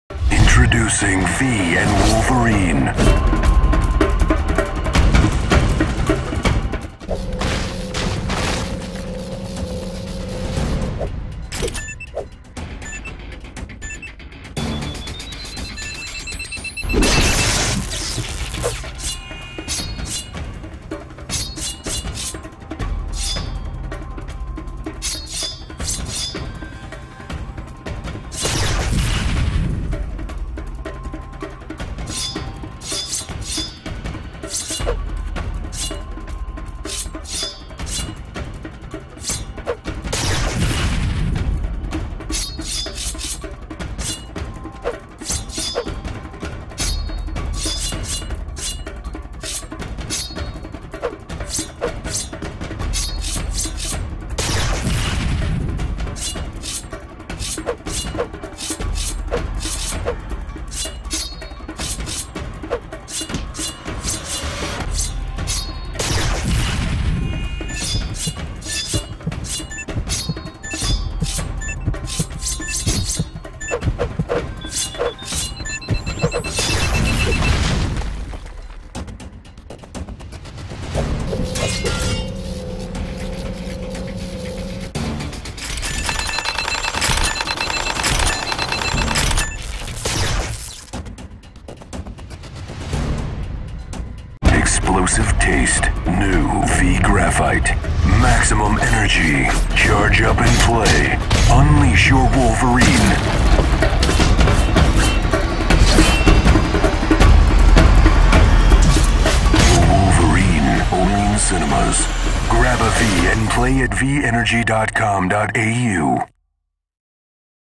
V Tokyo Fury Live - Rashman
Rashman dons the claws of Wolverine to battle Ninjas at Federation Square, Melbourne.

The Wolverine, Only in Cinemas